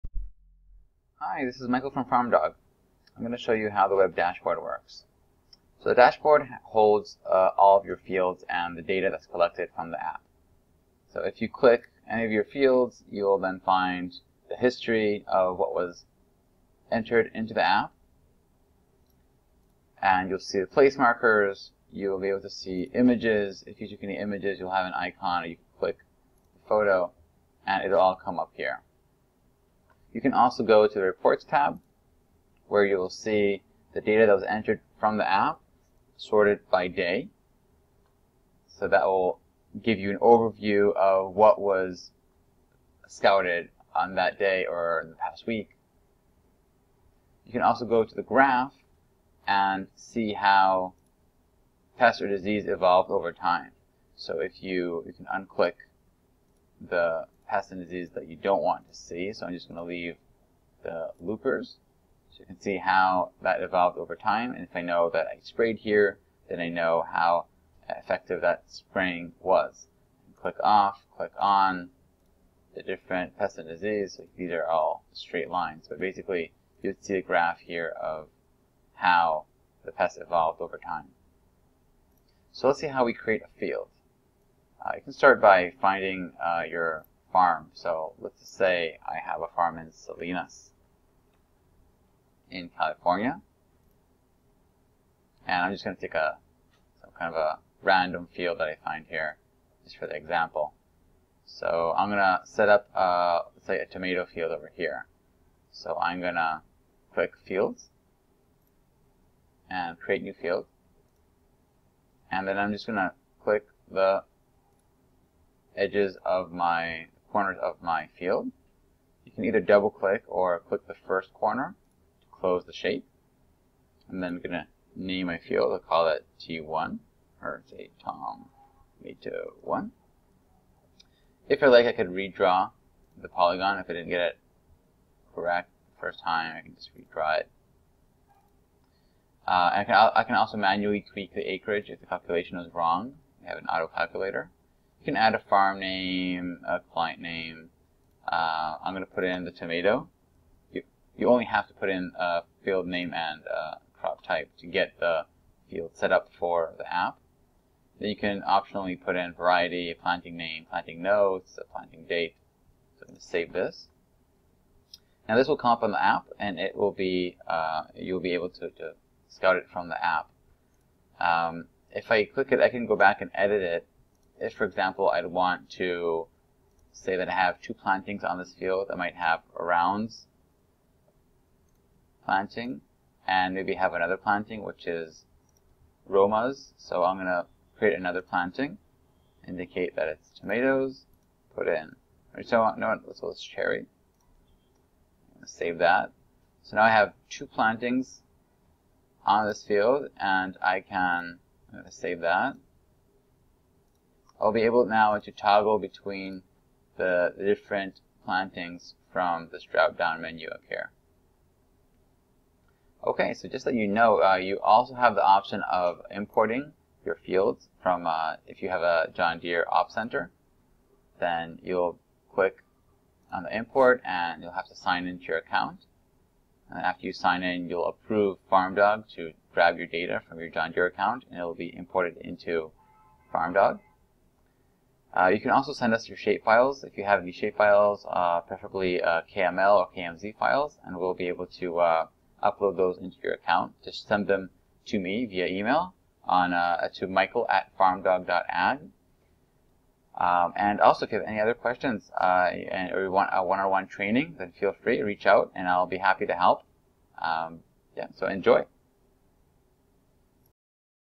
Farm Dog Scout - web dashboard tutorial
Learn how to use the Farm Dog web dashboard.

This tutorial covers:
- Viewing your scouted data and field history
- Setting up a new field
- Configuring an existing field
- Importing field data from John Deere